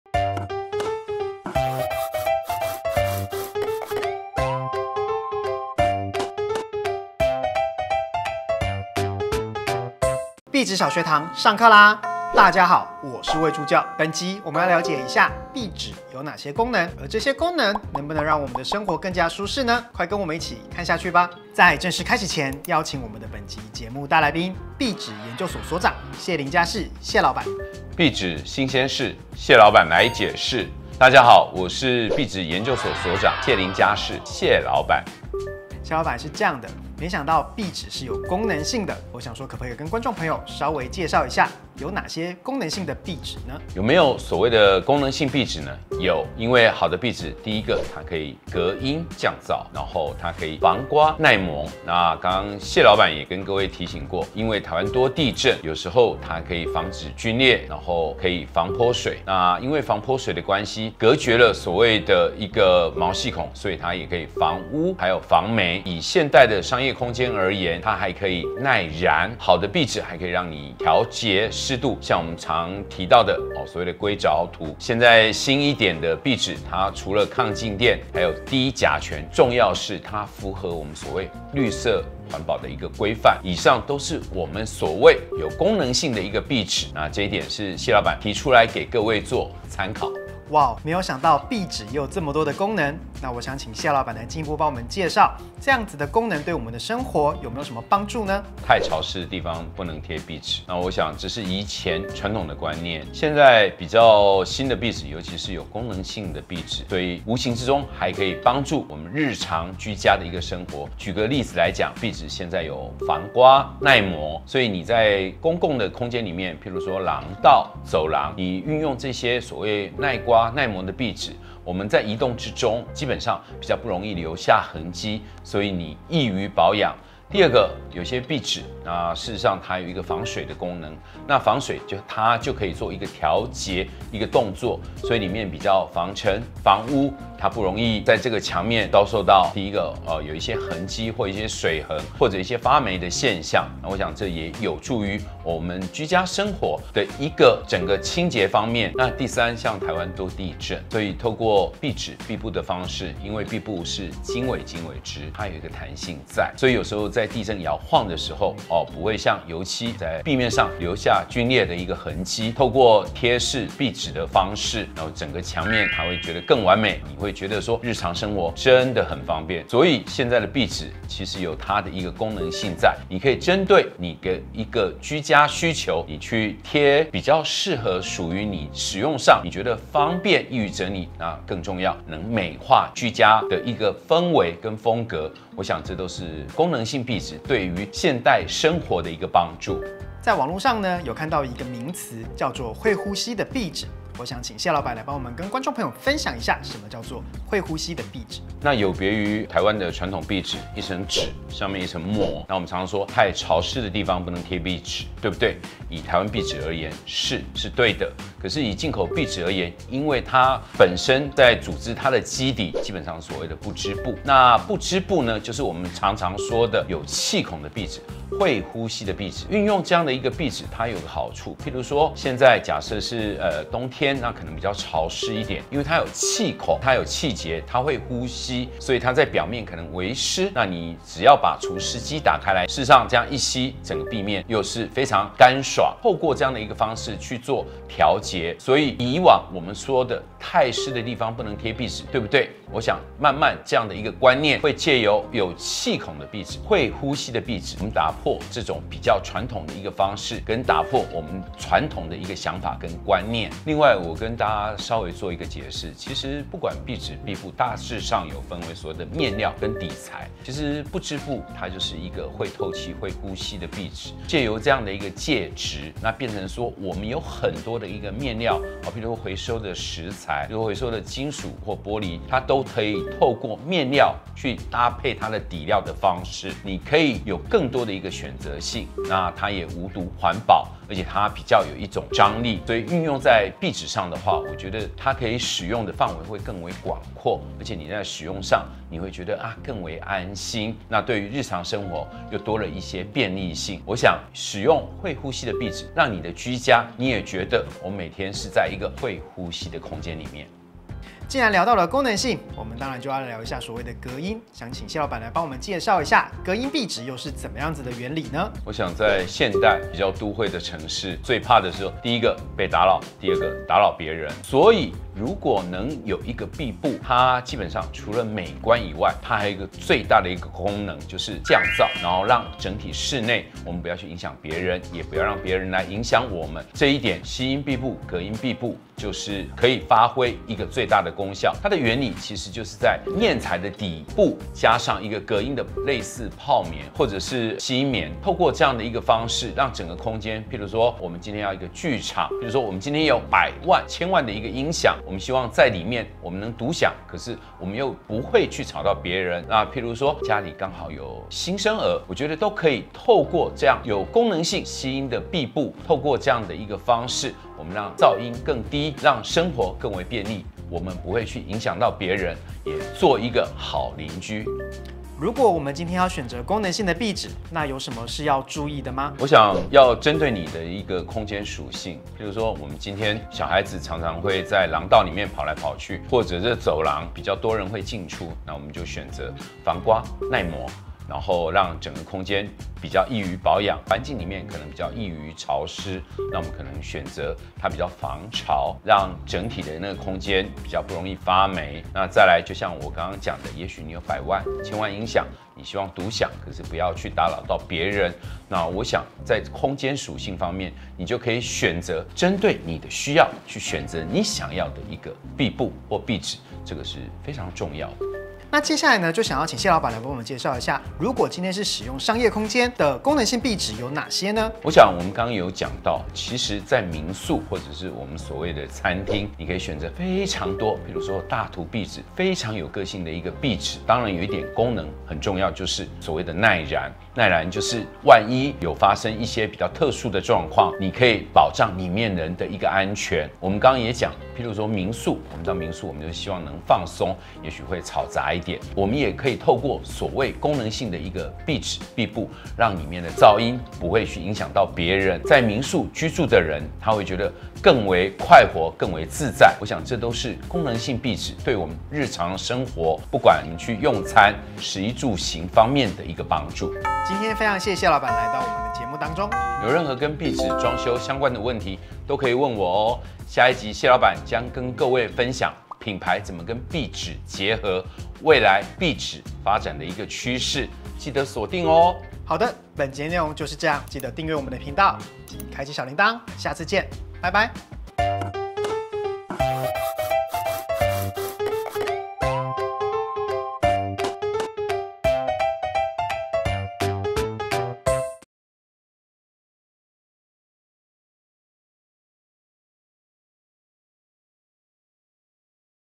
油漆不是唯一選擇！超越想像的多功能壁紙，不只防龜裂耐磨還有「這項」功能！有了它連隔音門窗都省了 Ft.榭琳傢飾 謝勝彰｜壁紙小學堂 ep.6/7
現在的多功能壁紙真的太神了！
不論是怕潮濕的衛浴空間，
或是需要安靜環境的視聽室，
降噪、隔音、耐燃、耐刮、防潑水都能一次到位，
真的是任何空間都適用，再也不用油漆了～

00:00｜影片開始
00:28｜客座教授介紹-壁紙研究所所長榭琳傢飾 謝勝彰
00:36｜Q：有哪些功能性壁紙？
01:31｜Q：功能性壁紙對生活的幫助？
02:33｜Q：風格與壁紙的搭配法則？
03:10｜Q：何謂會呼吸的壁紙？
05:01｜Q：壁紙為何可以隔音？
06:05｜Q：選購功能性壁紙要注意什麼？
06:48｜Q：商業空間要用什麼功能性壁紙？
07:37｜壁紙小學堂下課囉！